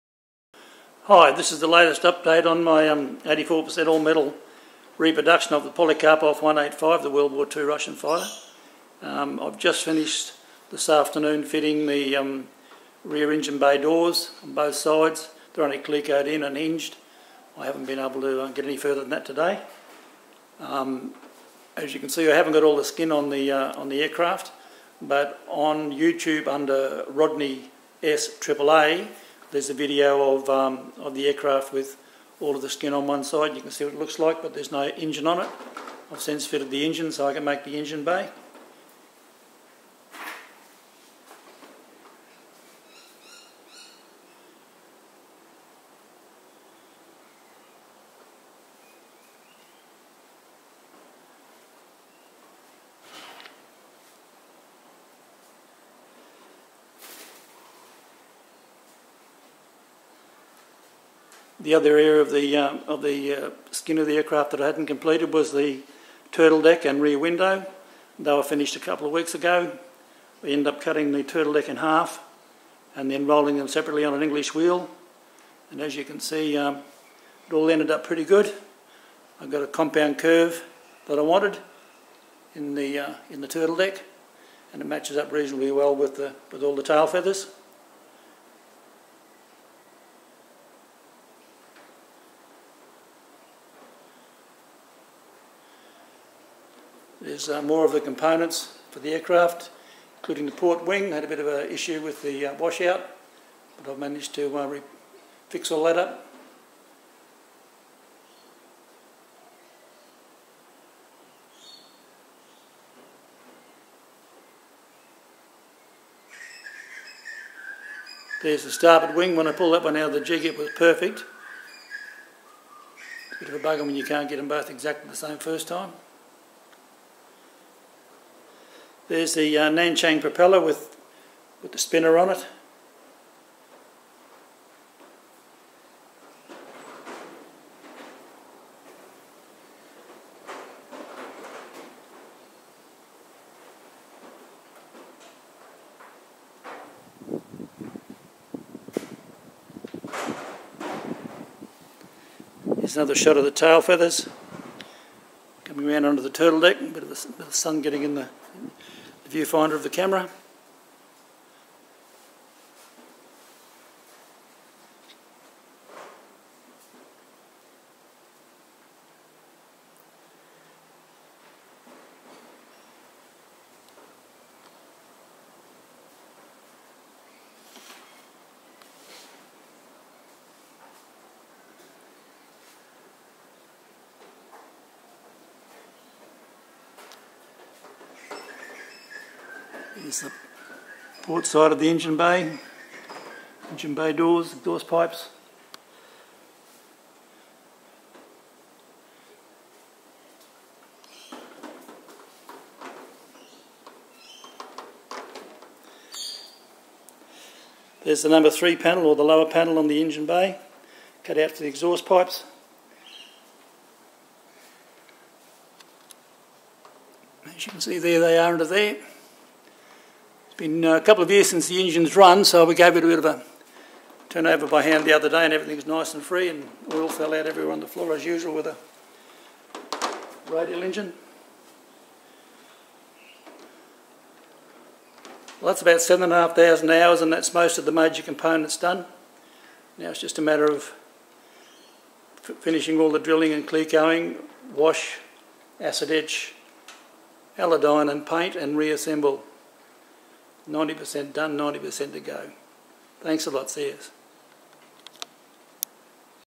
polikarpov 185 replica build update
this is video update of my replica polikarpov 185 russian ww2 fighter. This is an all metal 84% replica  with a nanchang 10 litre 9cyl radial engine 285hp . retractable U/C